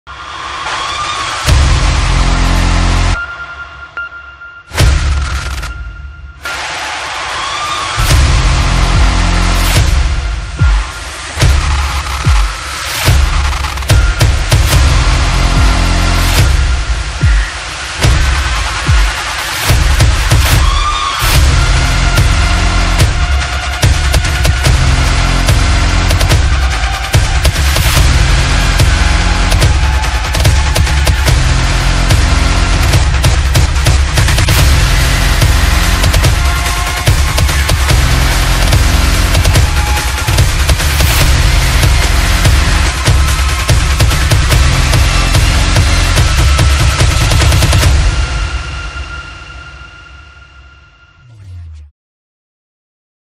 TOP50 DeutschRap 2020 [Trailer]
DeutschRapCharts Rückblick 2020 [TOP50]
Am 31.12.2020 wird ein Top50 Rückblick kommen :D
Welcher Künstler hat es nach eurer Meinung verdient in die Top50 für dieses Jahr zukommen ?
Eure Meinungen wären mal Interessant :D
Draußen wird man nicht Knallen dürfen also lassen Wir alle es mal hier Knallen :D